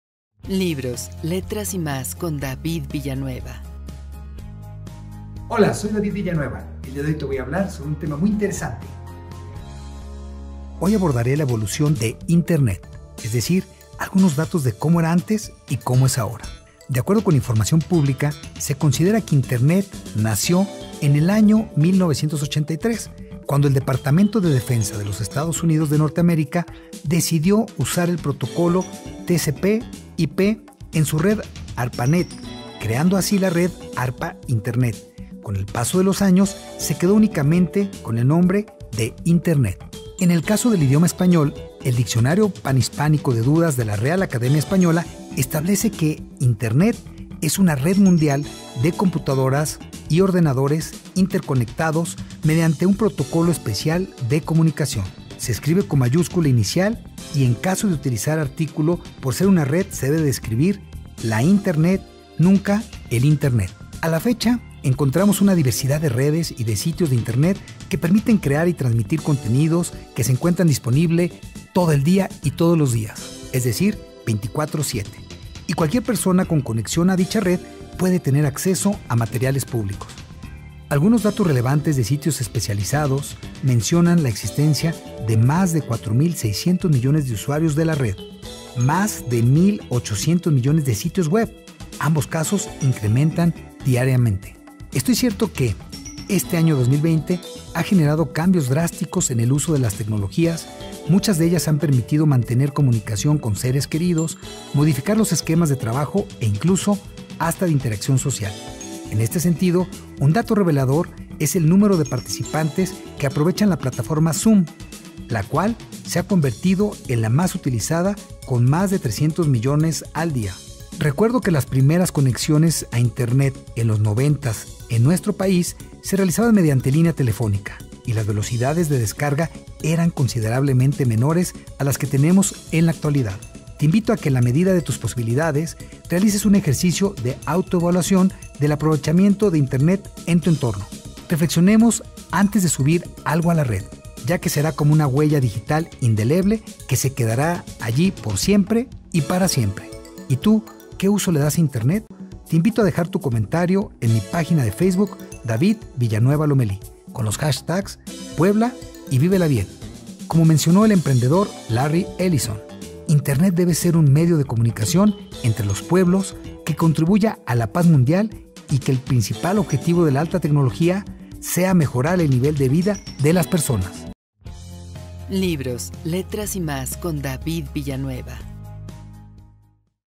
Internet ayer y hoy
Sin duda, uno de los elementos que caracterizó la evolución en el siglo XX y los inicios del actual, es el relacionado con los avances tecnológicos. Hoy en Libros, letras y más, te platico sobre la evolución de Internet, un espacio cada vez más globalizado. Y tú, ¿qué beneficios has identificado con su uso? Compárteme tu comentario.